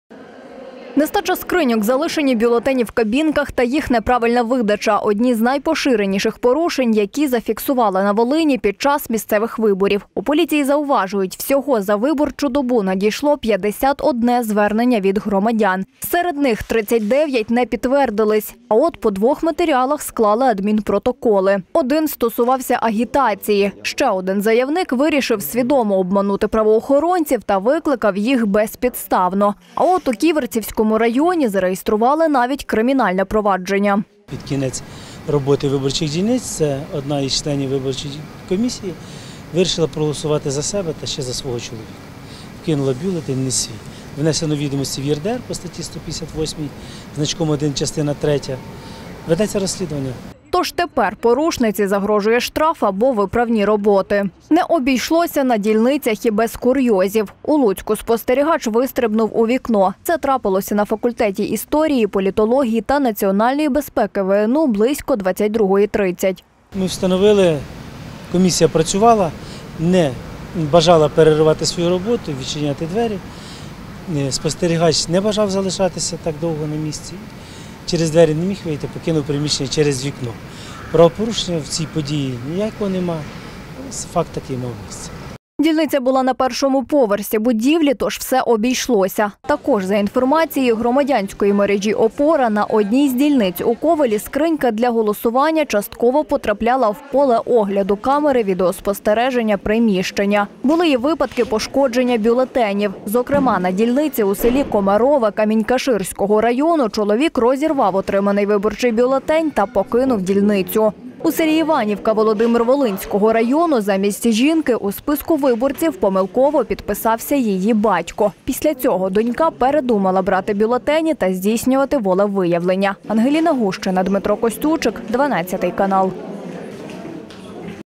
Виборчі порушення
На Волині під час місцевих виборів громадянська мережа "ОПОРА" нарахувала чимало дрібних порушень